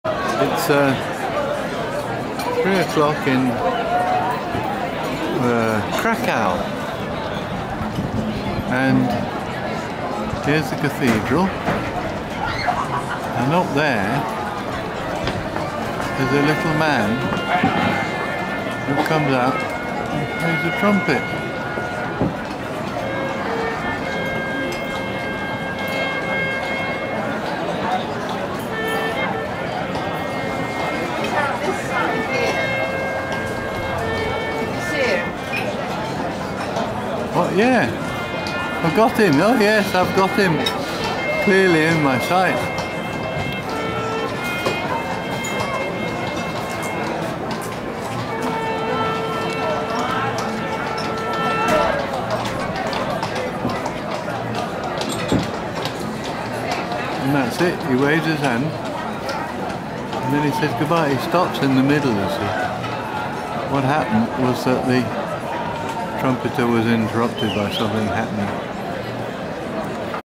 St Mary's Trumpeter Krakow
Every hour a trumpet signal is played from the top of one of the towers.